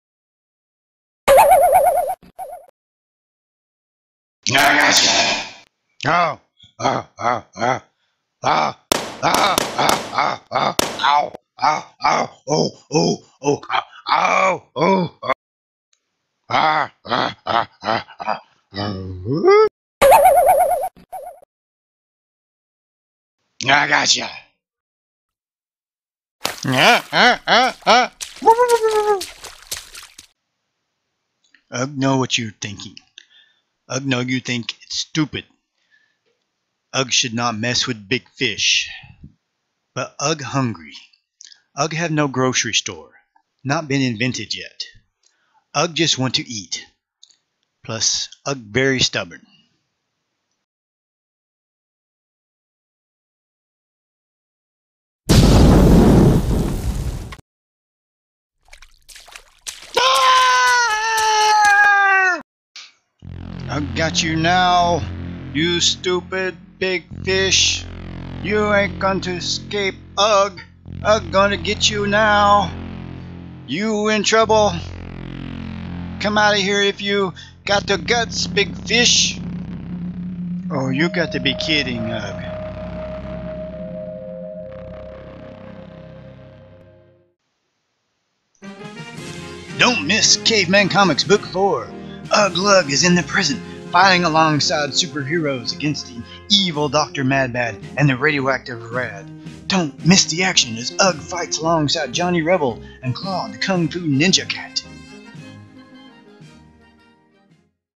Caveman Comics: Ug Vs Big Jaws Cartoon
Ug Lug the caveman is hungry again. This time he's going fishing instead of hunting. This time around Ug faces a large and angry fish with a bad attitude, Big Jaws! Can Ug Survive? Will the shark eat Ug?

Don't miss Ug's latest adventures in Caveman Comics Book Four on sale at Amazon.com today.